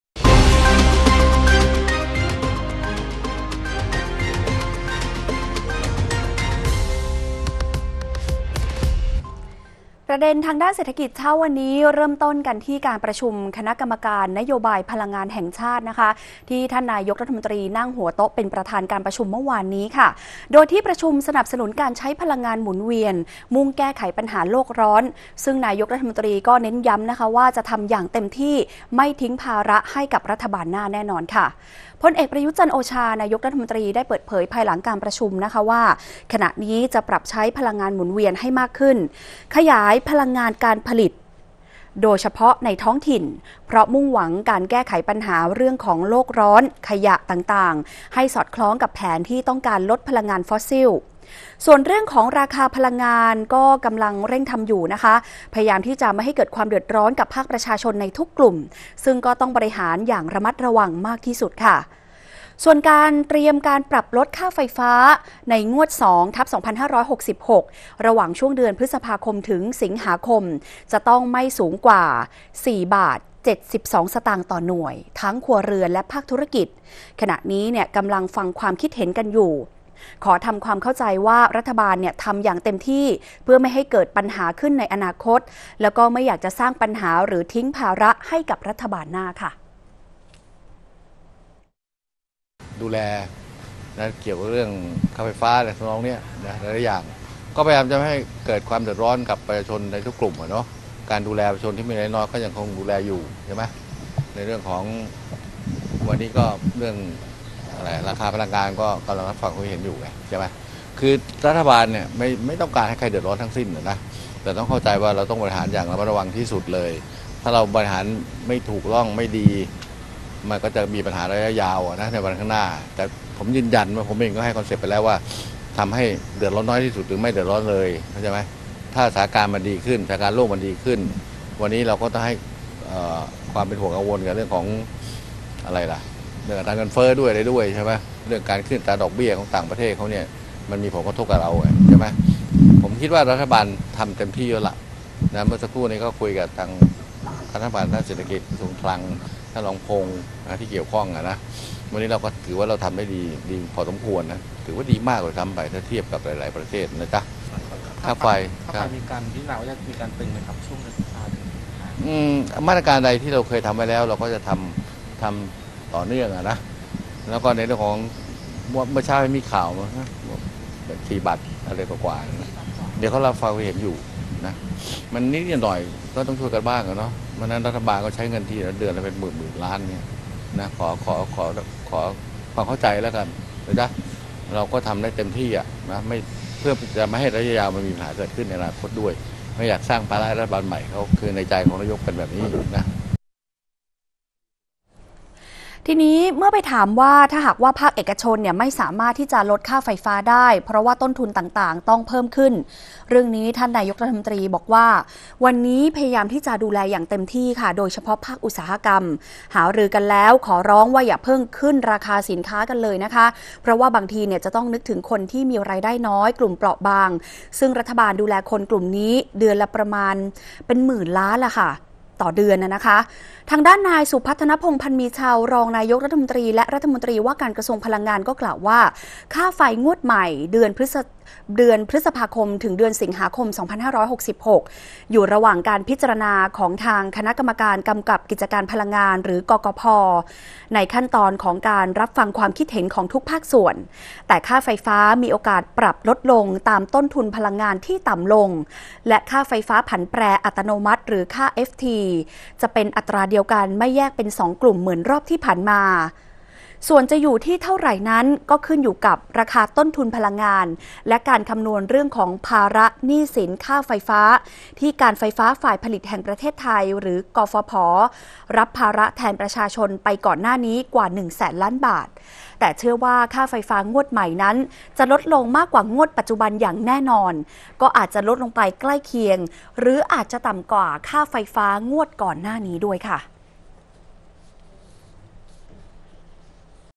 เตรียมปรับลดค่าไฟงวดใหม่ (พ.ค. - ส.ค.) ข่าวเช้า วันที่ 10 มีนาคม 2566
ประเด็นเศรษฐกิจเช้านี้ เริ่มที่การประชุมคณะกรรมการนโยบายพลังงานแห่งชาติ ที่นายกรัฐมนตรี เป็นประธานการประชุมเมื่อวานนี้ โดยที่ประชุมสนับสนุนการใช้พลังงานหมุนเวียน มุ่งแก้ปัญหาโลกร้อน ซึ่งนายกรัฐมนตรี ก็ย้ำว่า จะทำอย่างเต็มที่ ไม่ทิ้งภาระรัฐบาลหน้า